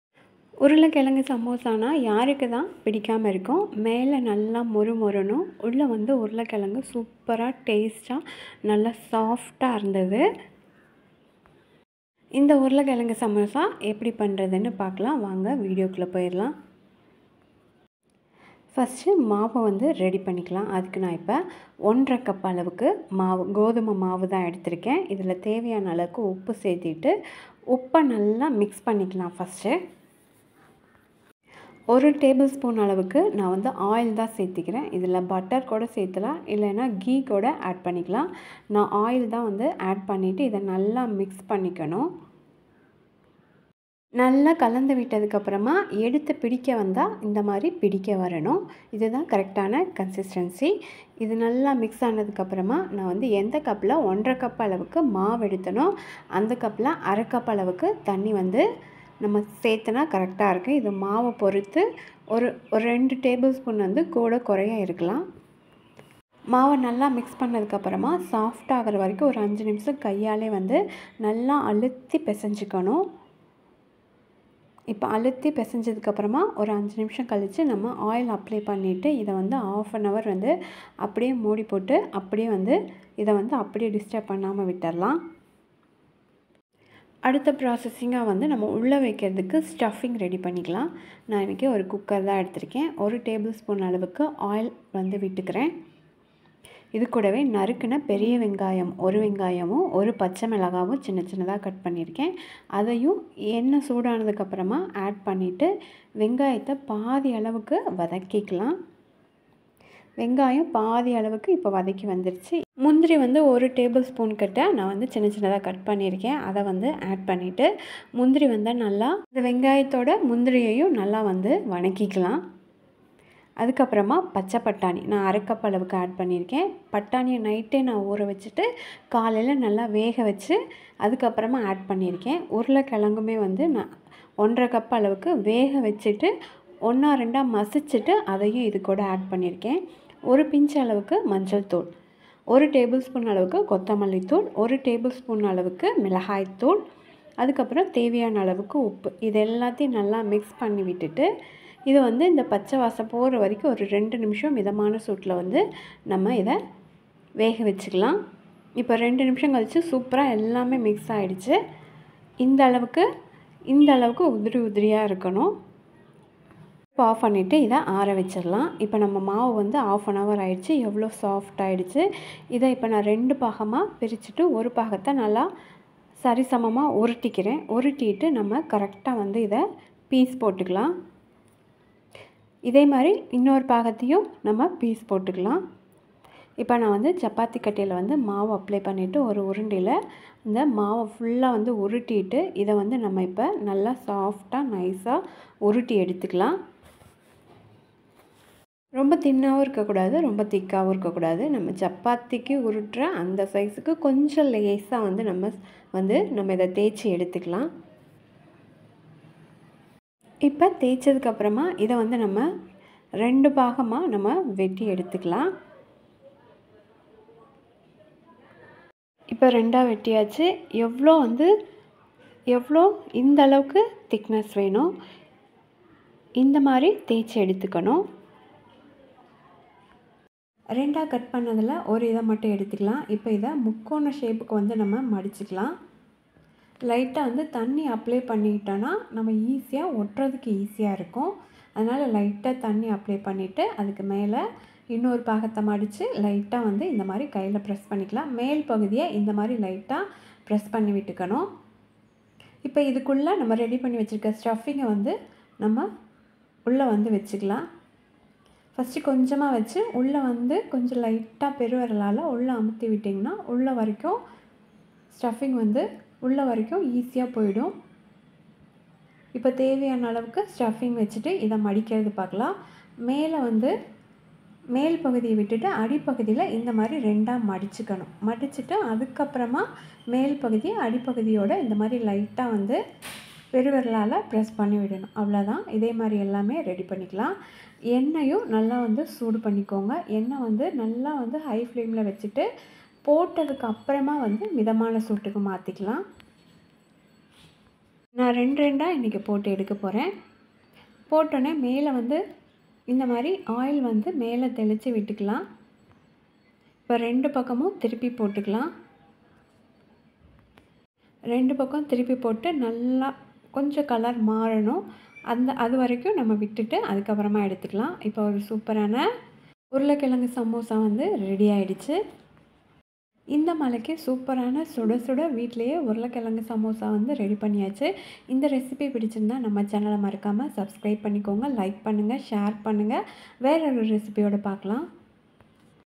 Potato Samosa Recipe/Aloo samosa recipe/Snacks Recipe
தேவையான பொருட்கள்
கோதுமை மாவு-1 1/2கப்
என்ணெய் -1 டேபிள்ஸ்பூன்
உப்பு தேவையான அளவு
தண்ணீர்-அரை கப்

மசாலா
உருளைக்கிழங்கு 1 1/2கப்
பச்சை பட்டாணி 1/2கப்
வெங்காயம்-1
பச்சை மிளகாய்-1
மஞ்சள் தூள்-1 பின்ச்
கொத்தமல்லித்தூள்-1 டேபிள்ஸ்பூன்
மிளகாய்த்தூள்-1 டேபிள்ஸ்பூன்
என்ணெய்-1 டேபிள்ஸ்பூன்

பொரித்து எடுக்க
என்ணெய்-தேவையான அளவு

Ingredients
Wheat flour-1 1/2cup
Oil-1 tablespoon
Salt -required
Water-1/2cup

Stuffing
Oil-1 tablespoon
Big onion-1
Green chilli-1
Greenpeas -1/2cup
Potato -1 1/2cup
Turmeric-1 pinch
Salt-required

Try this recipe at home and comment in video